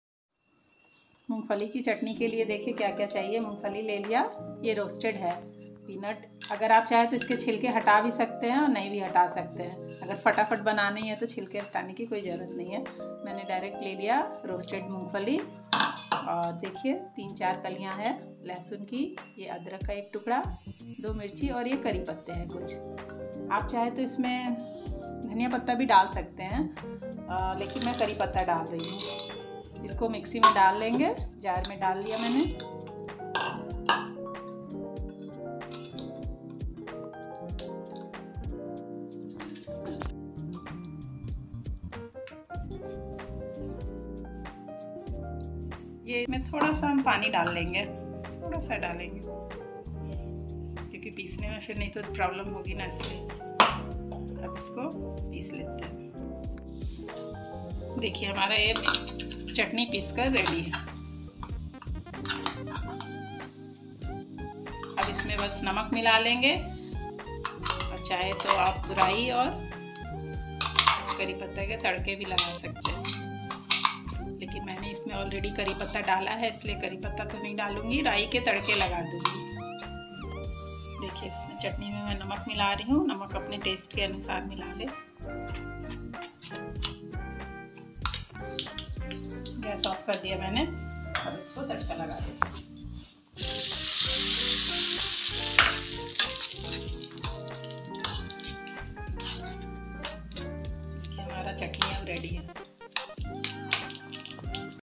How to make peanut chutney/मूंगफली की मजेदार चटनी बनाये बहुत ही जल्दी और कम सामानों से ।
मूंगफली की मजेदार चटनी बनाये बहुत ही जल्दी और कम सामानों से।